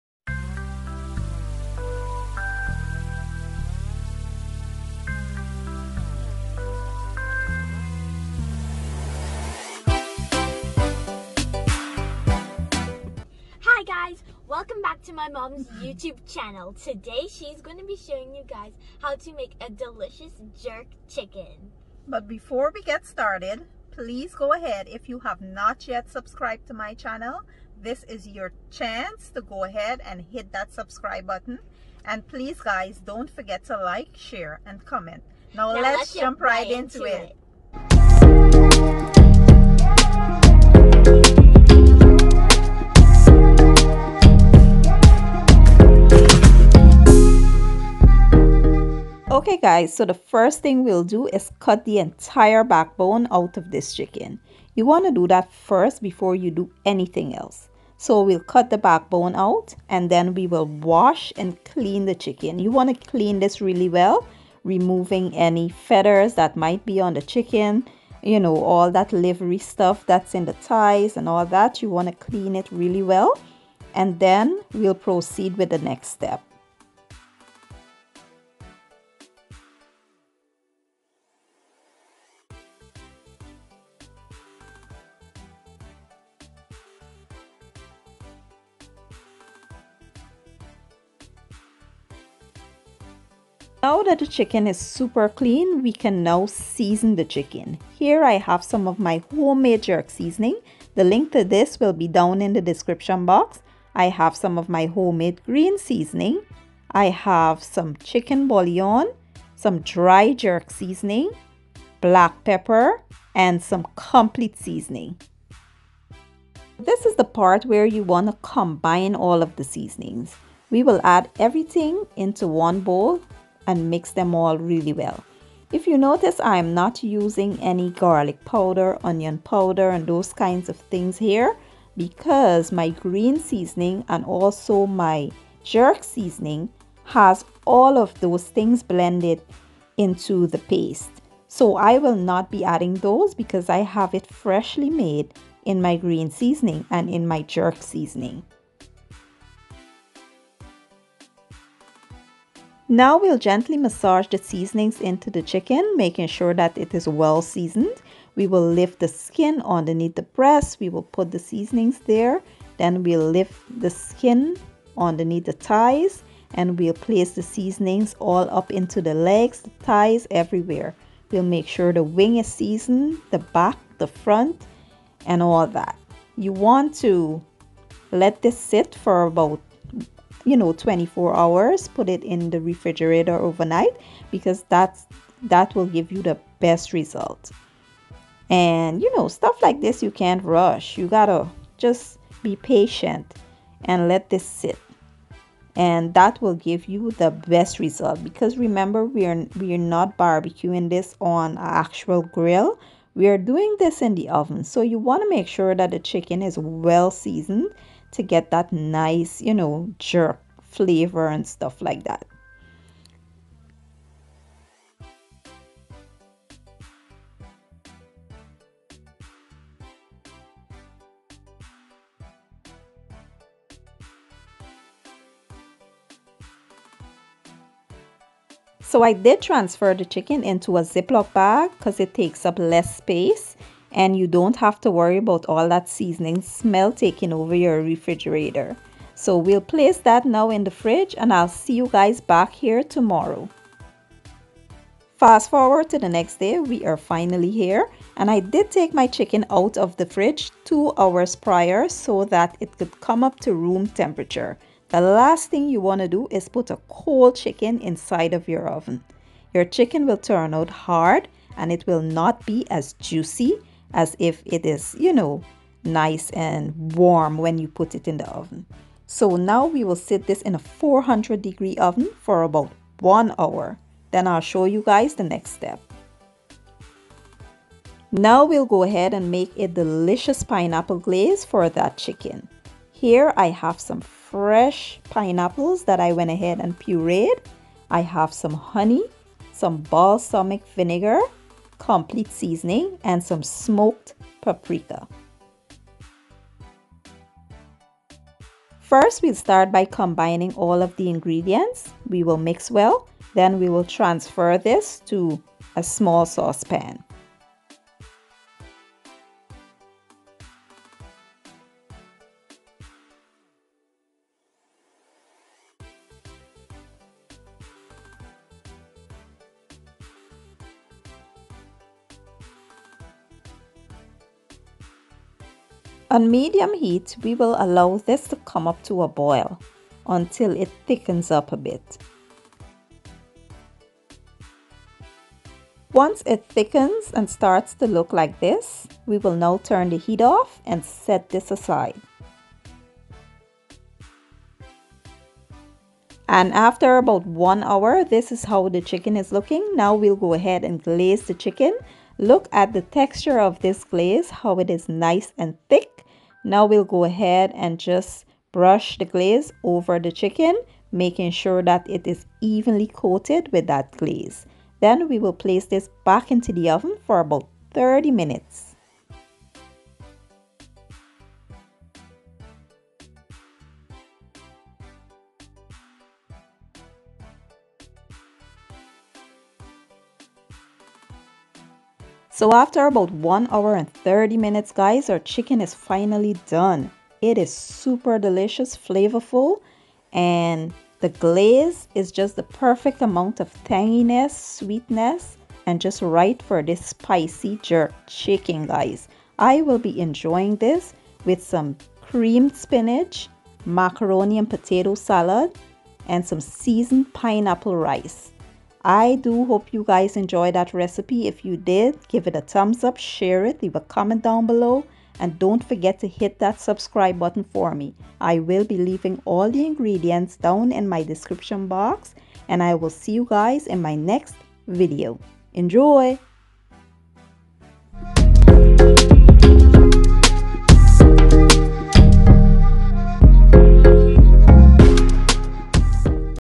PINAPPLE GLAZED JERK CHICKEN| recipe
3 lb whole chicken
1/2 cup jerk seasoning
2 tbsp green seasoning
1 tbsp chicken bouillon
1 tsp complete seasoning
1 tbsp dry jerk seasoning
1/2 tsp black pepper

glaze

1/2 cup fresh pureed pinapple
1/2 cup honey
1/4 cup balsamic vinegar
1 tsp complete seasoning
1/2 tsp smoked paprika